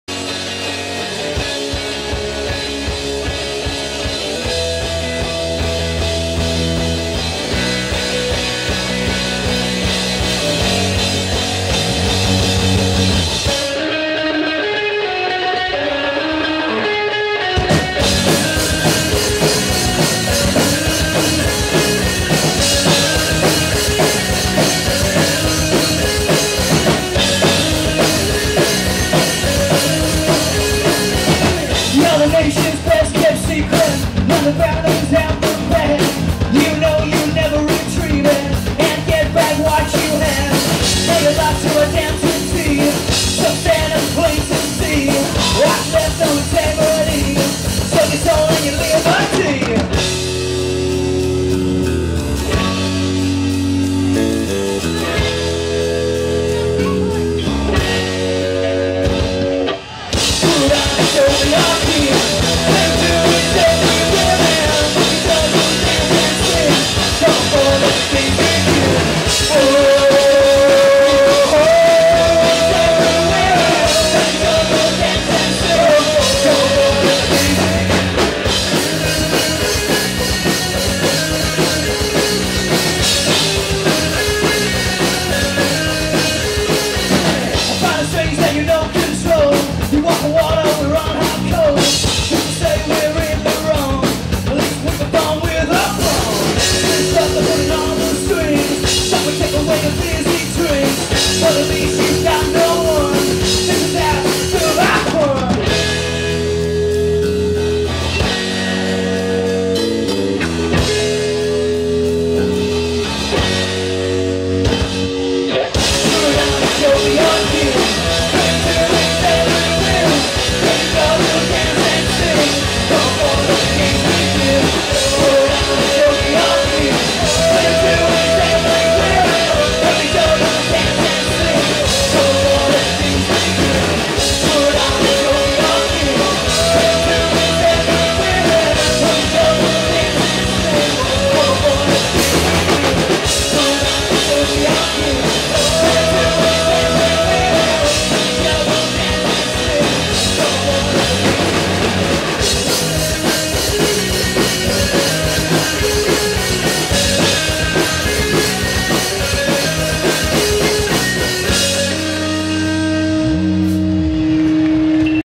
Electric Youth Revolt - Ipswich - Music In The Park 025
Electric Youth Revolt did a great set on the Ipswich Community Radio Stage at This years 20th anniversary of Ipswich Music Day, one of the largest free one-day music events in the UK. The event features the brightest talent across Suffolk and East Anglia, spanning almost every type of music and performing across six stages in the park throughout the day